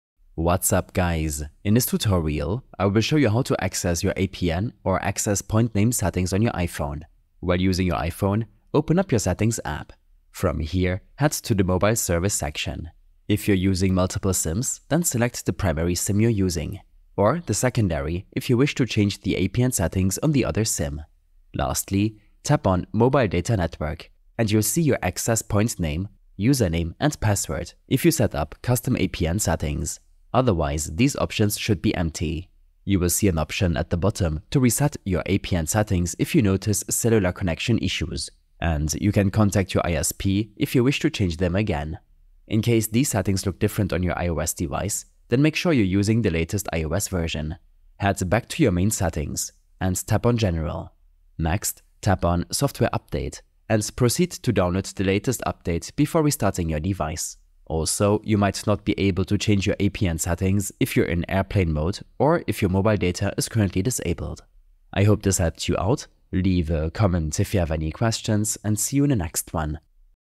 How To Access APN Settings on iPhone - Tutorial
A short tutorial on accessing your APN, or access point name settings on your iPhone.